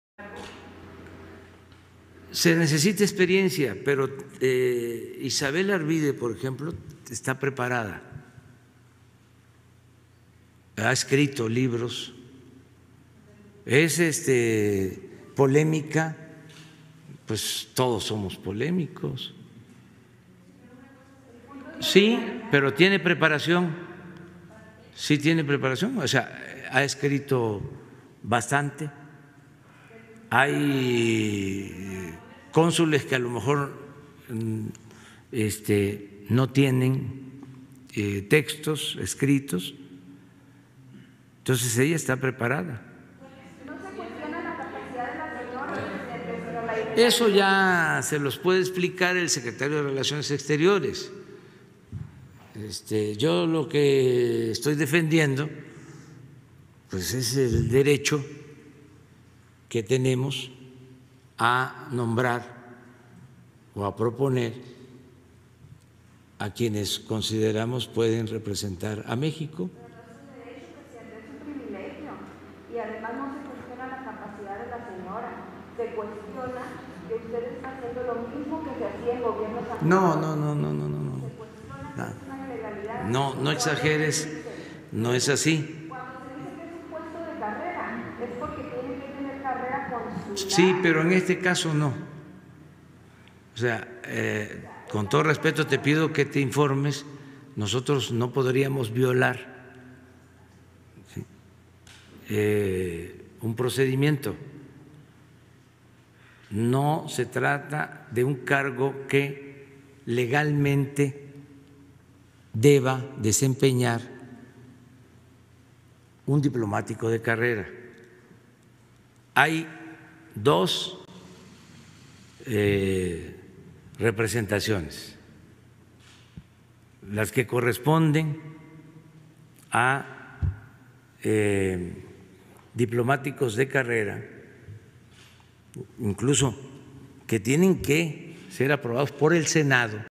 LA APLAUDIDORA "ISABEL ARVIDE" AMLO dice que está preparada
dice que Isabel Arvide está preparada para ser cónsul porque ha escrito libros y que hay cónsules que no lo han hecho.